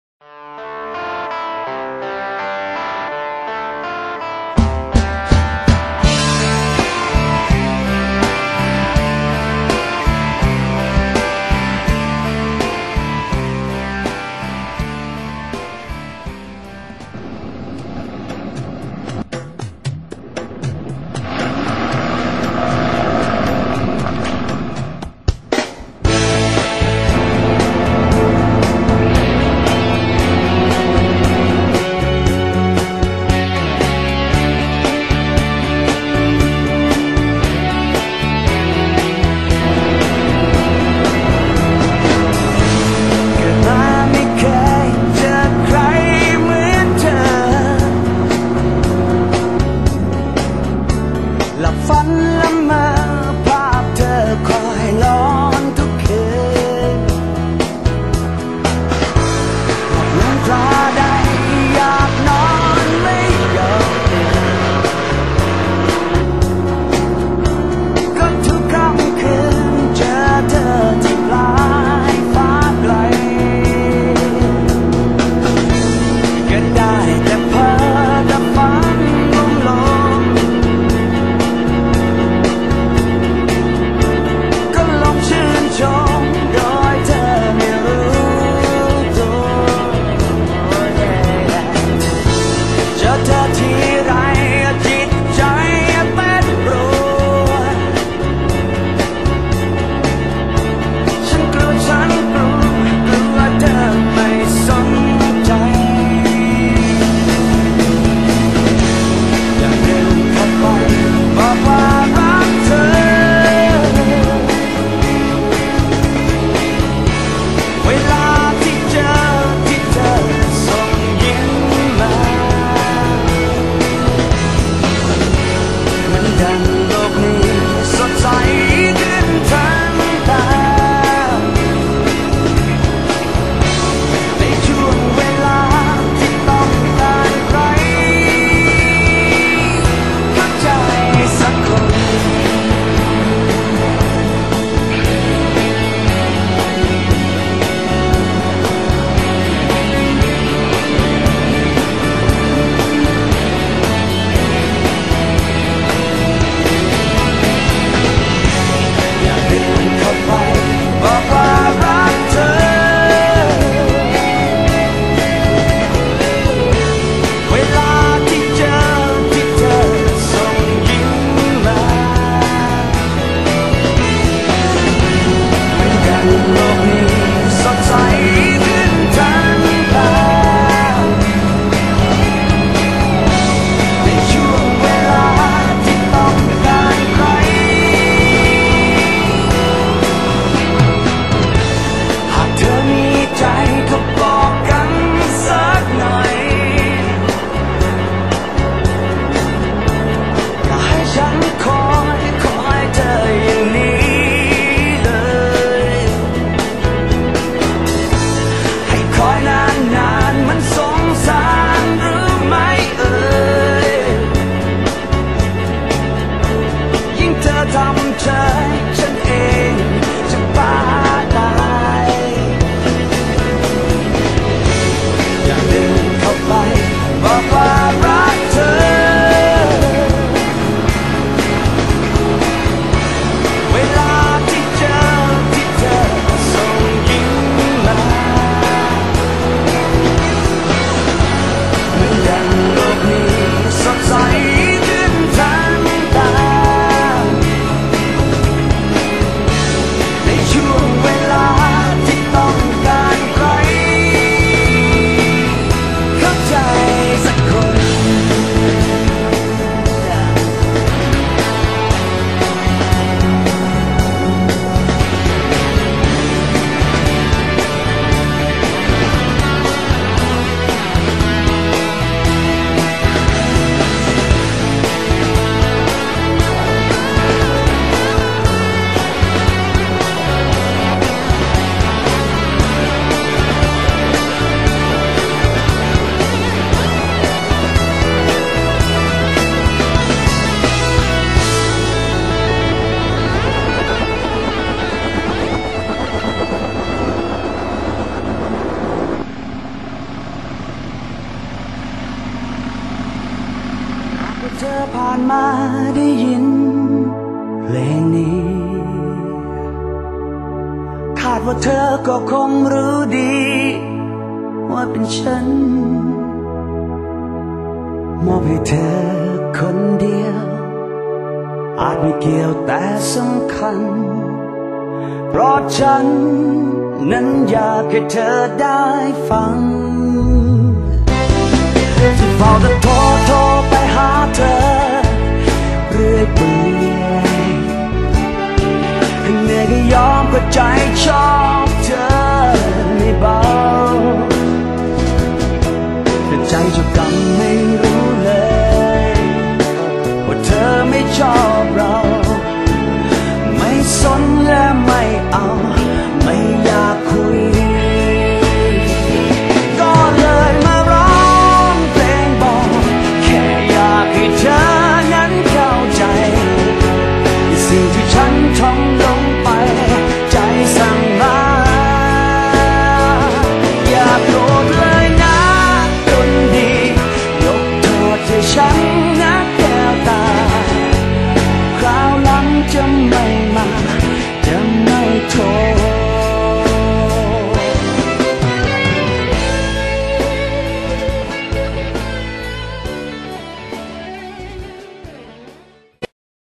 The Gyroplane2
Flying in the gyroplane, Thai music, location spain, pilot and camera from UK